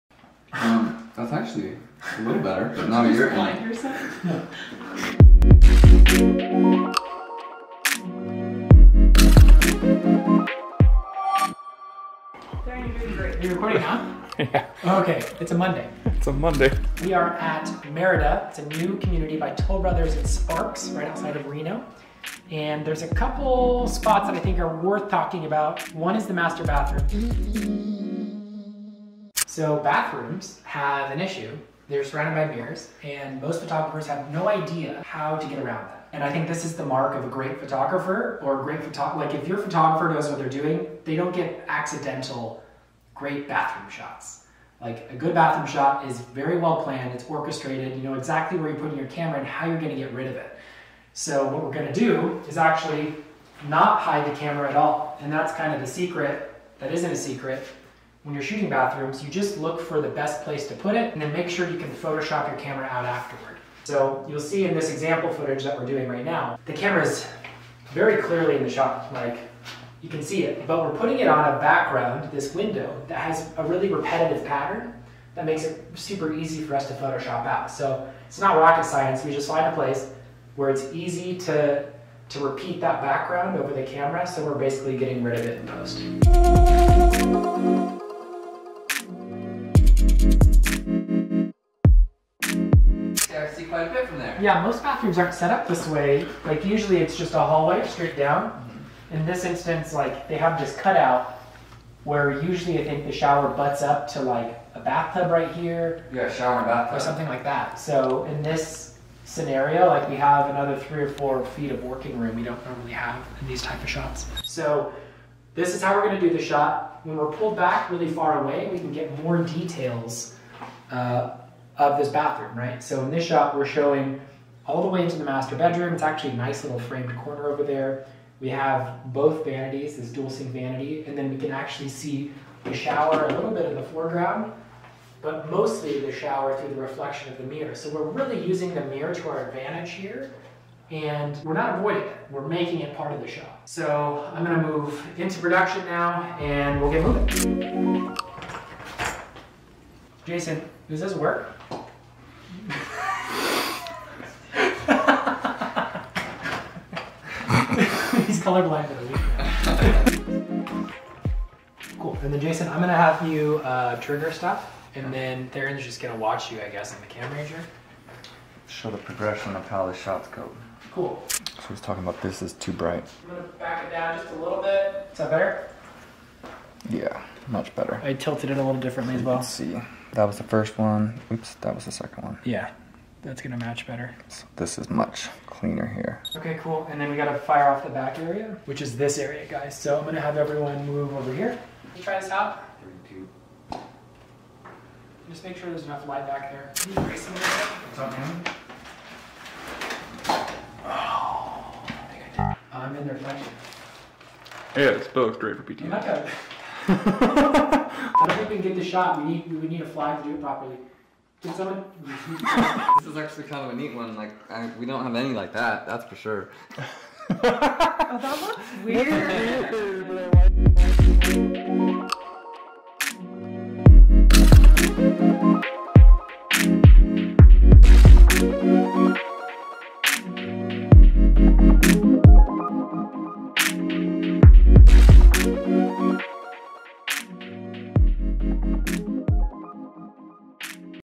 How To Photograph Bathrooms With Mirrors
In this episode of behind the scenes with Davies Imaging Group, Chad discusses the issue(s) surrounding photography within bathrooms. The main culprit - Mirrors. How do we get that perfect shot?

Learn how YOU can take the perfect shot, and get a quick glimpse of this gorgeous bathroom by Toll Brothers in their new Belvior model at Merida!